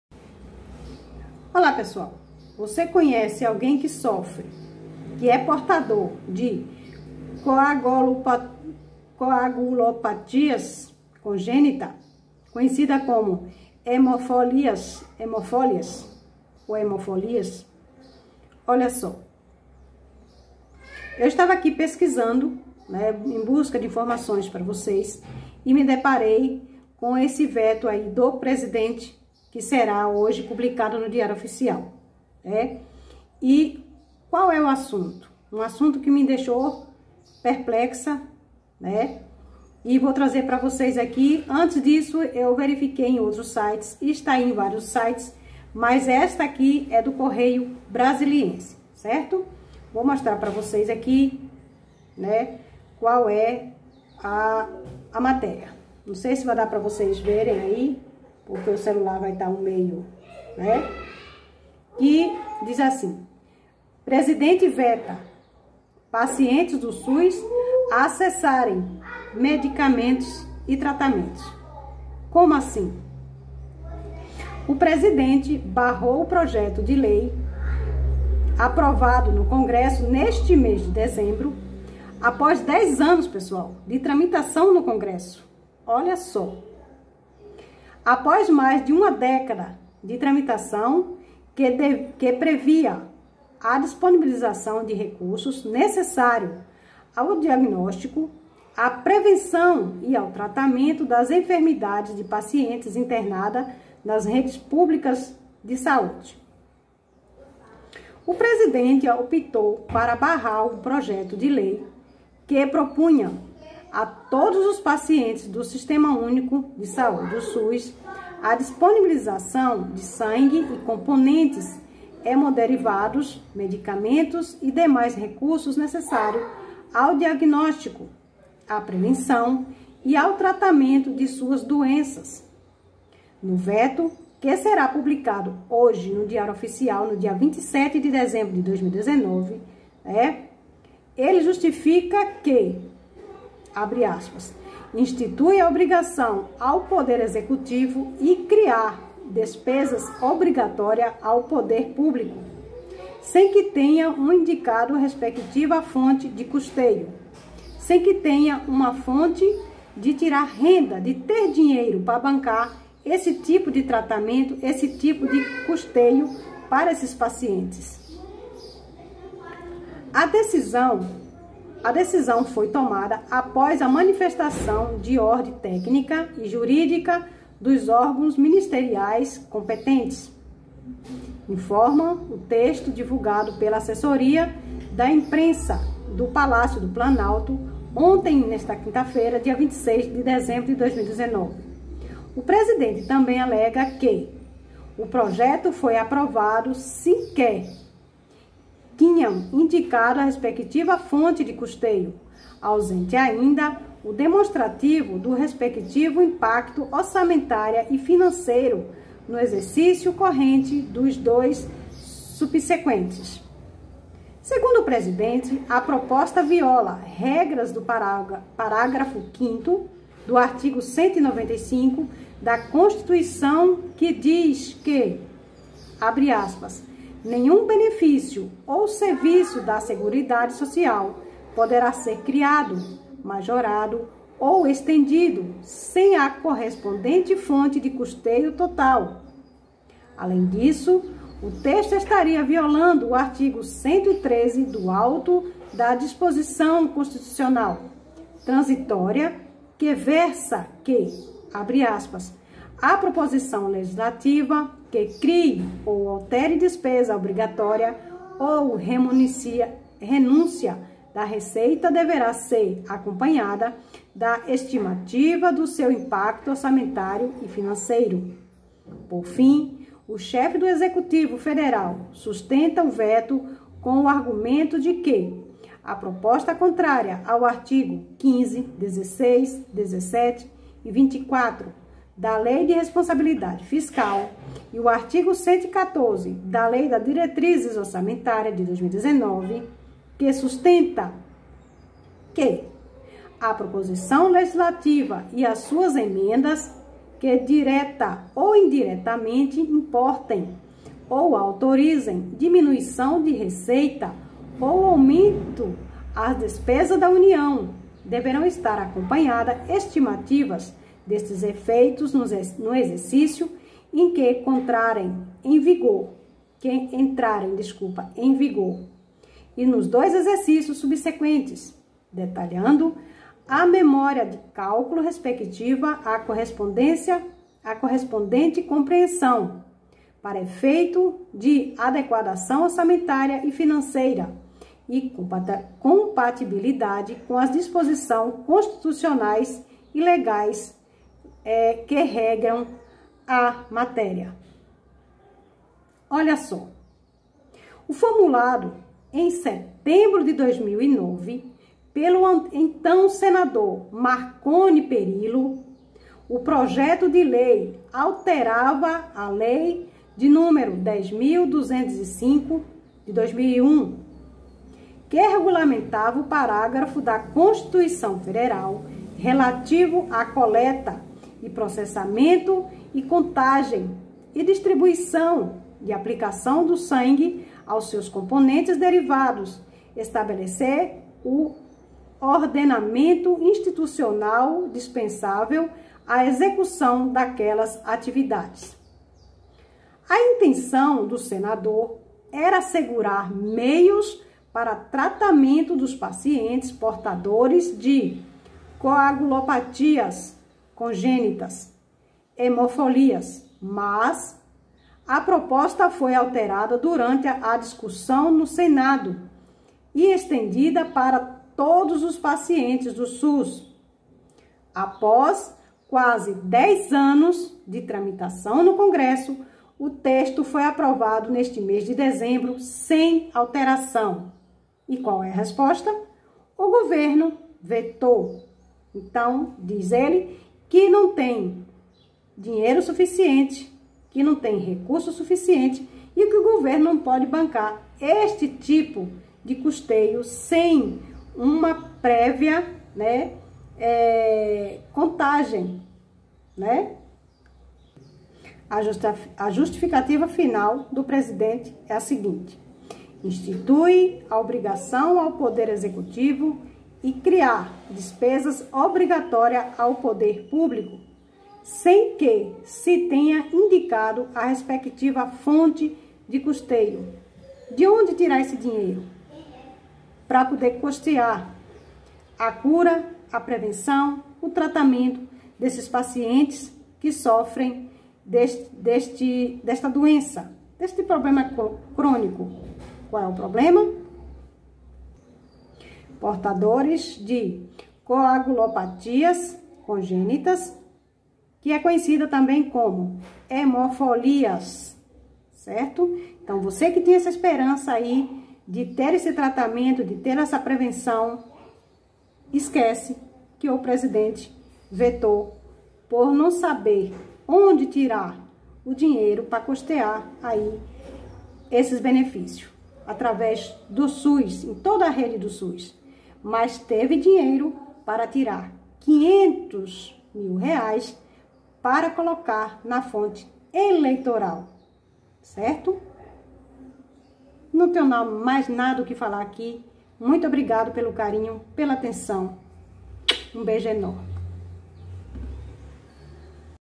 Governo vetou projeto que da direito pacientes garantir tratamentos e medicamentos do SUS.
Não há dúvida de que está forma de tratamento para pacientes com Coagulopatias Crônicas ( hemofilias) não será objeto de governo federal.